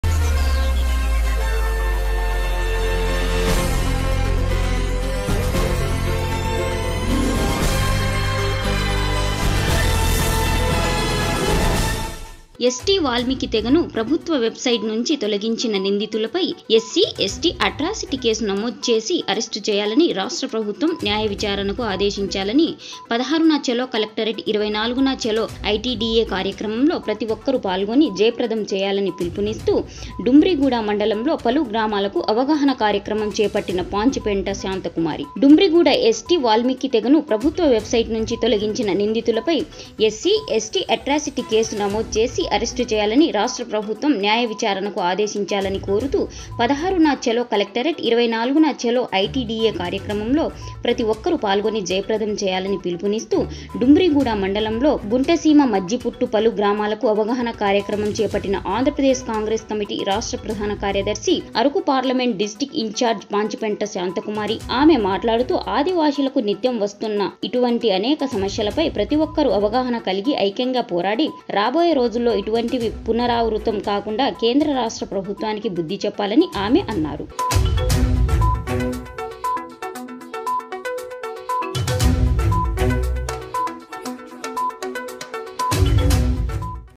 16,న చలో కలెక్టరేట్, 24న చలో ఐ.టీ.డీ.ఏ జయప్రదం చేయాలి
డుంబ్రిగుడా న్యూస్ తేదీ: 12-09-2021 ఎస్టి వాల్మీకి తెగను ప్రభుత్వ వెబ్ సైట్ నుంచి తొలగించిన నిందితులపై ఎస్సీ ఎస్టీ అట్రాసిటీ కేసు నమోదు చేసి అరెస్టు చేయాలని రాష్ట్ర ప్రభుత్వం న్యాయ విచారణకు ఆదేశించాలని కోరుతూ 16,న చలో కలెక్టరేట్ 24న చలో ఐటీడీఏ కార్యక్రమంలో ప్రతి ఒక్కరూ పాల్గొని జయప్రదం చేయాలని పిలుపునిస్తూ డుంబ్రిగుడా మండలంలో గుంటసీమ , మజ్జిపుట్టు పలు గ్రామాలకు అవగాహన కార్యక్రమం చేపట్టిన ఆంధ్రప్రదేశ్ కాంగ్రెస్ కమిటీ రాష్ట్ర ప్రధాన కార్యదర్శి అరకు పార్లమెంట్ డిస్ట్రిక్ట్ ఇంచార్జ్ శ్రీమతి పాచిపెంట శాంతకుమారి.  ఆమె మాట్లాడుతూ ఆదివాసీలకు నిత్యం వస్తున్న ఇటువంటి అనేక సమస్యలపై ప్రతి ఒక్కరు అవగాహన కలిగి ఐక్యంగా పోరాడి రాబోయే రోజుల్లో ఇటువంటివి పునరావృతం కాకుండా కేంద్ర రాష్ట్ర ప్రభుత్వానికి బుద్ధి చెప్పాలని ఆమె అన్నారు. ఈ కార్యక్రమంలో అరకు పార్లమెంట్ నియోజకవర్గం యూత్ కాంగ్రెస్ అధ్యక్షుడు మొస్య ప్రేమ్ కుమార్  కమ్మిడి హరీష్ కుమార్, శరభ గంగా, దశయ్య  కాంగ్రెస్ పార్టీ అధ్యక్షులు కార్యదర్శులు కార్యకర్తలు అభిమానులు మహిళలు యువత పాల్గొనడం జరిగింది*_